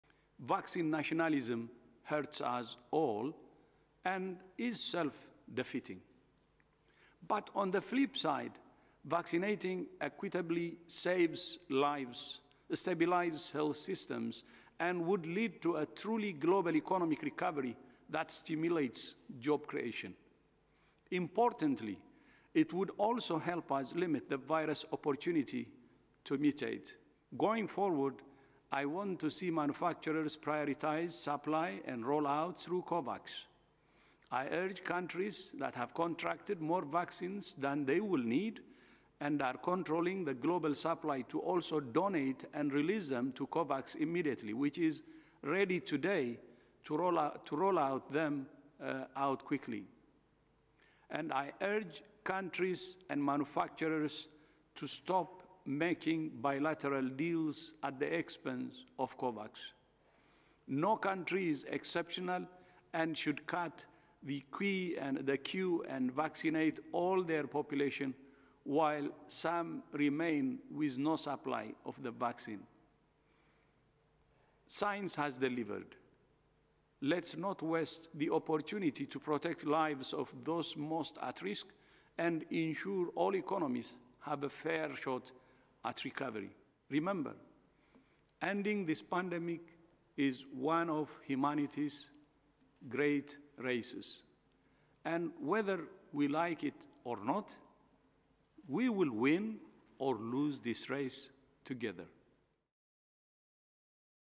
COVID-19 vaccines - The time to deliver vaccines equitably is now
WHO Director-General Dr Tedros: "I urge COVID-19 vaccine manufacturers to prioritise supply through COVAX; countries that have contracts for more vaccines than they need should donate them to COVAX immediately and stop making bilateral deals. COVAX is ready.  The time to deliver vaccines equitably is now!"